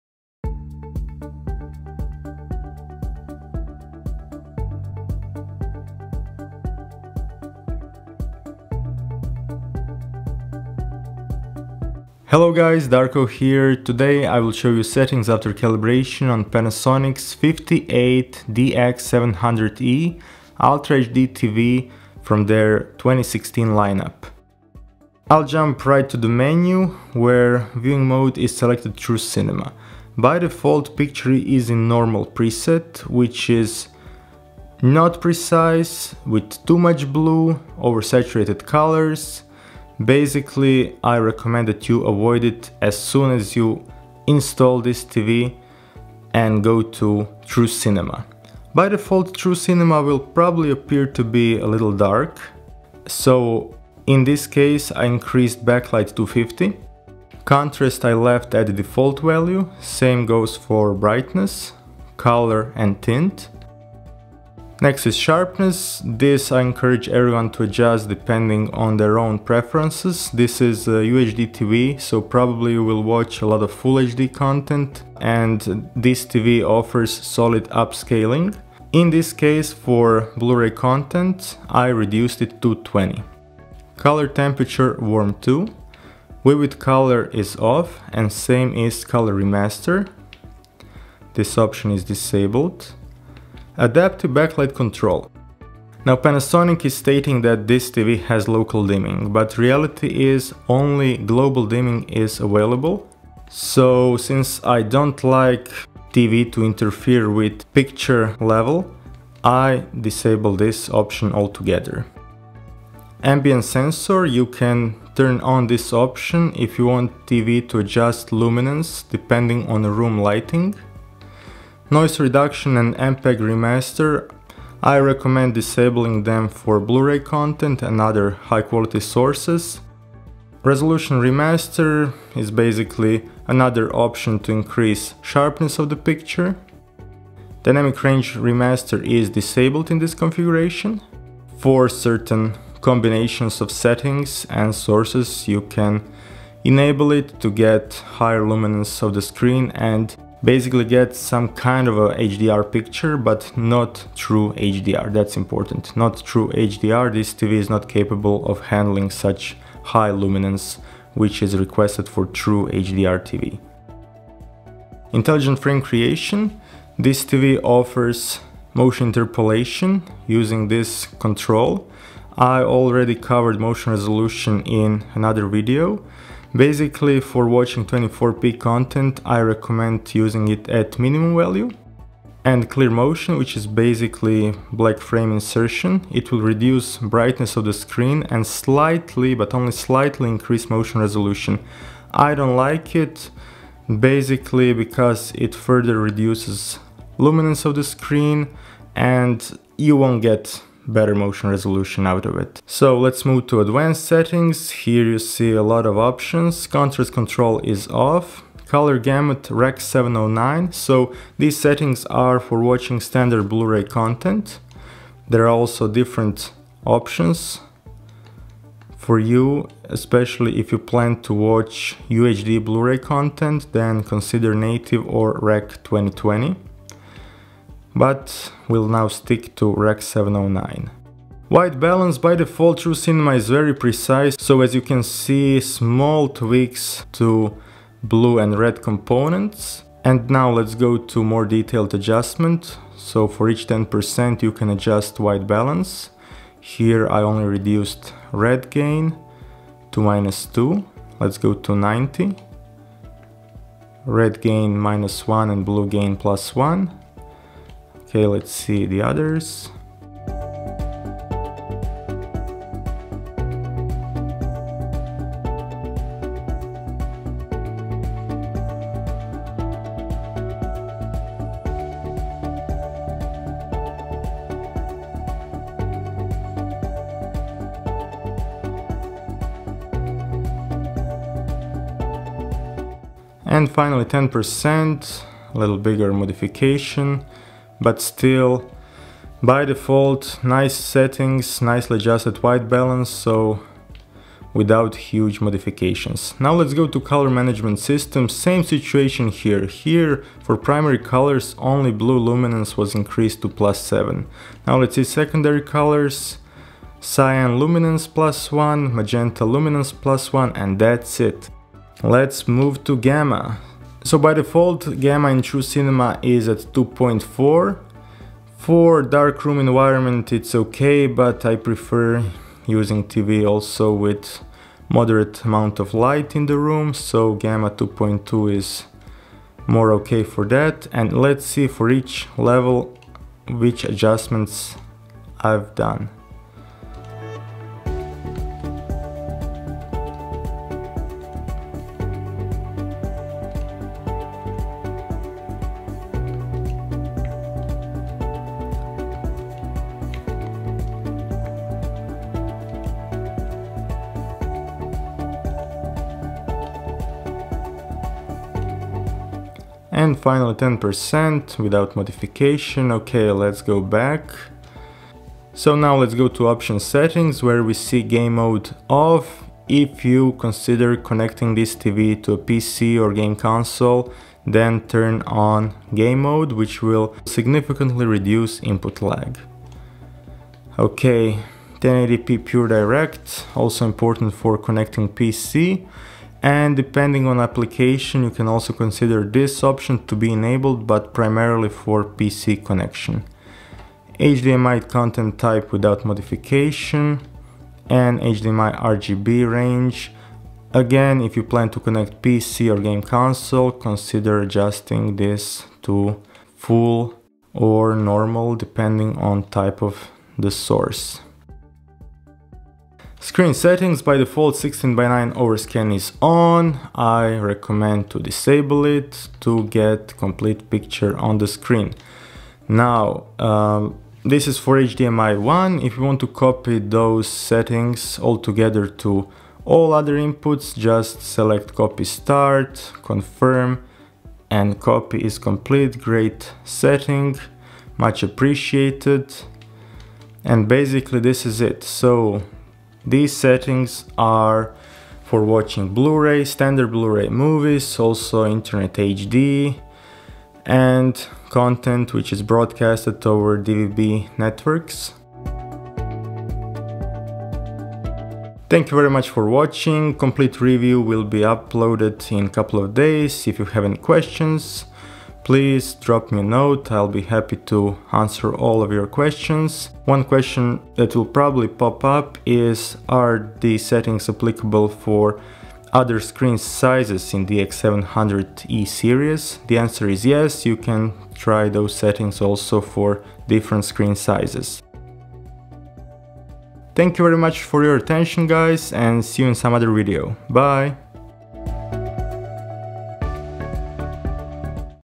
Panasonic DX700 DX700E settings after calibration
Panasonic DX700E Ultra HD TV calibrated settings for Blu-ray, internet HD video and other content with Rec.709 color gamut.

NEW!

Peak luminance: 136 nits
Gamma: 2.2
Colorspace: Rec.709